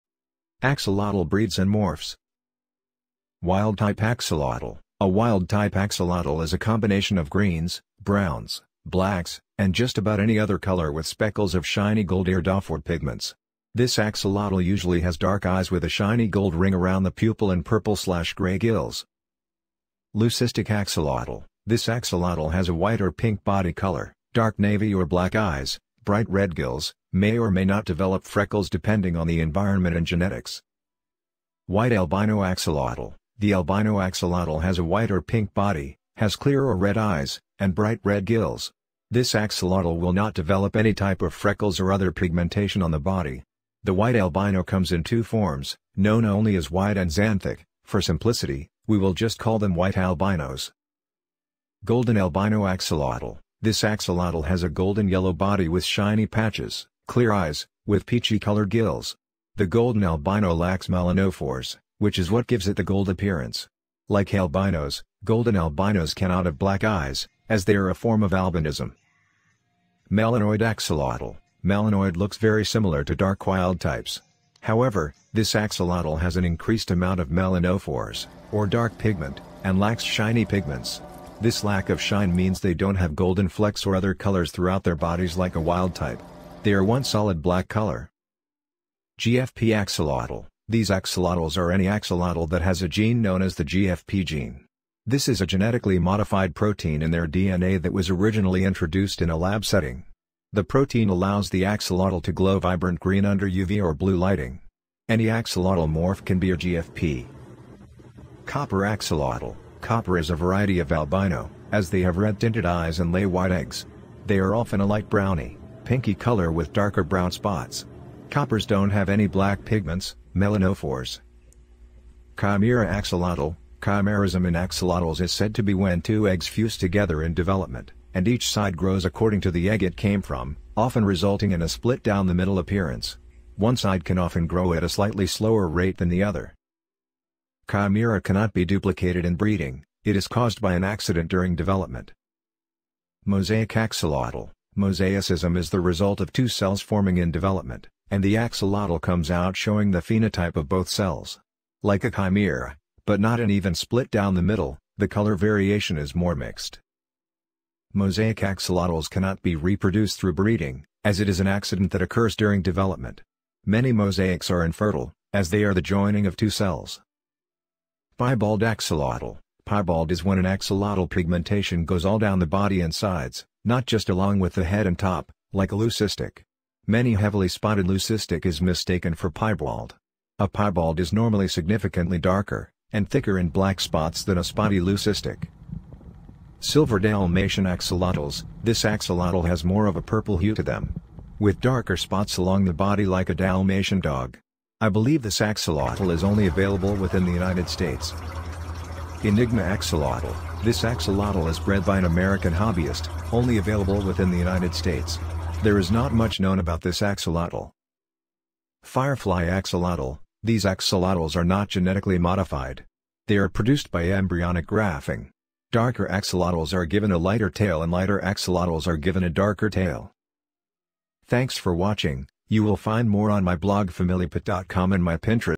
All the Axolotl Breeds , Colors & Morphs [ Videos ]
Axolotl breeds , Colors & Morphs
There are generally 5 main ( melanoid ( Melanistic ) , leucistic , wild , the Golden and The Albino )  types of Axolotls, for the colors it is a little difficult to list them all but we can classify them according to these categories:

Leucistic
Golden Albino
Wild Type axolotls
Piebald axolotl - rare color
Mosaic axolotls -rare morph
Copper axolotl
Lavender or Silver Dalmatian axolotls - rare ones too
Black Melanoid
White Albino
Speckled Leucistic
Rare -Chimera axolotl
Heavily-Marked Melanoid
GFP Axolotls or Green Fluorescent Protein
Firefly axolotls , from Indiana but rare ones too
Enigma Axolotl - very rare axolotls